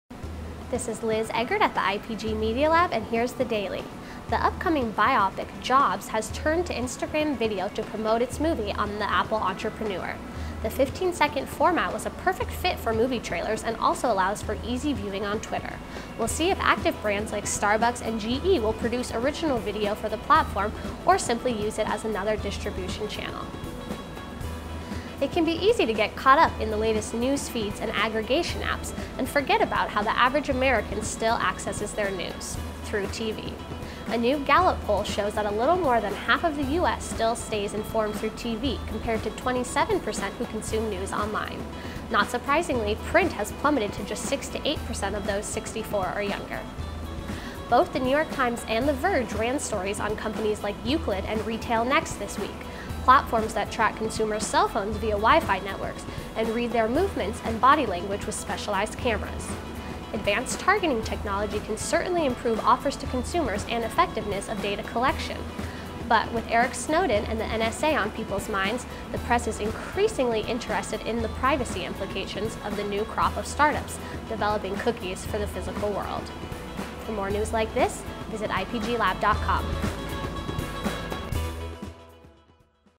The Trigger 7/16/13 - IPG Media Lab
All your ad tech news in 90 seconds or less. Delivered Monday through Friday at the IPG Media Lab in New York.